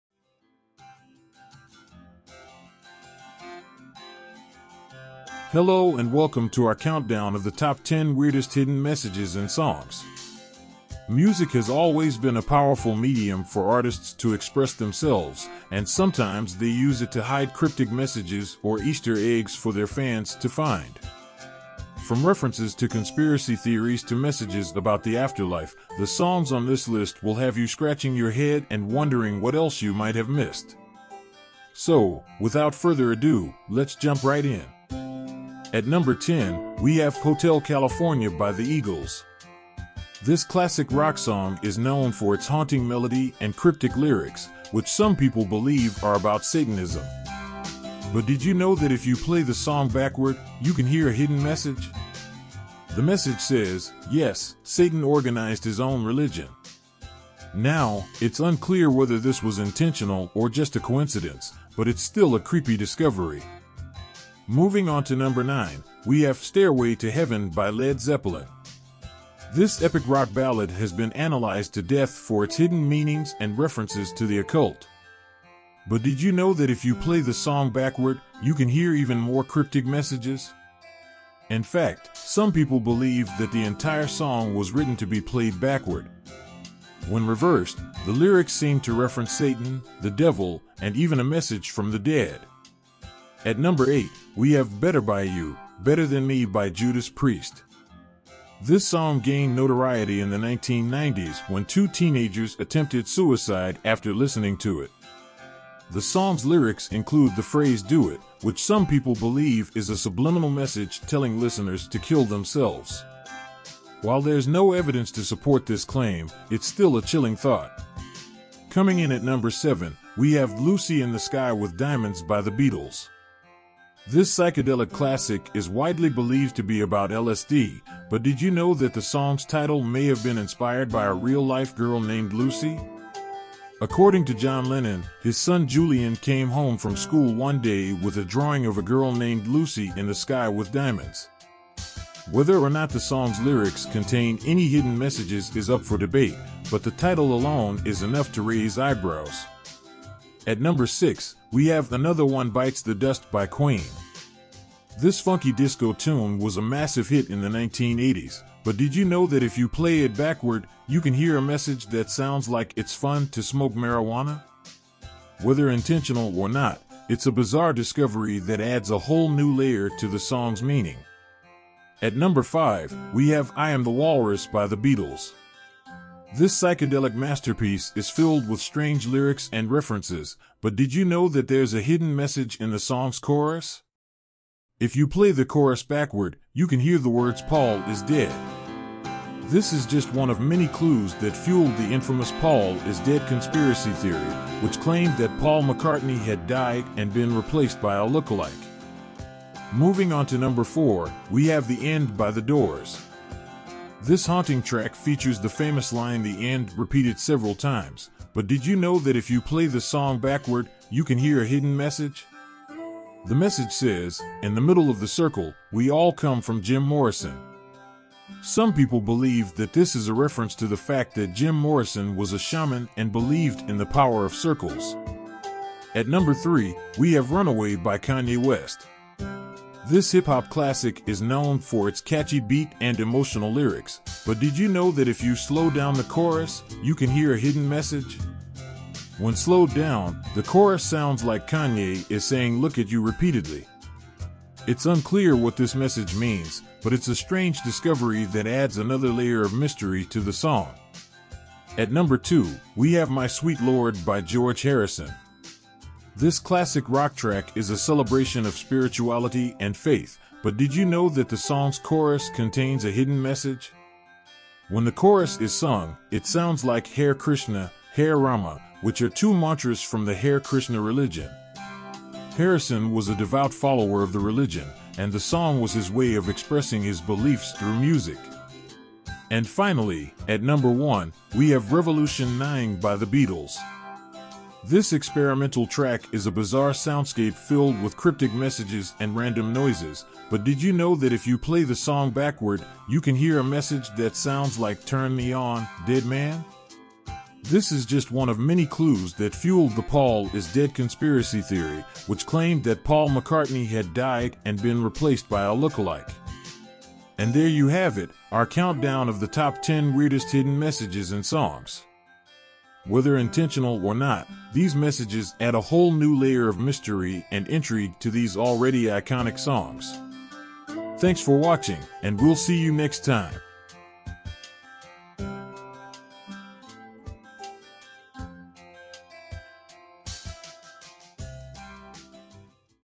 Unbelievable! These 10 Creepy Hidden Messages in Famous Songs Will Shock You!
Here is a look at the top 10 creepiest hidden messages in famous songs that you never knew about. You will be shocked at some of the artists included from Kanye West to Queen and many more.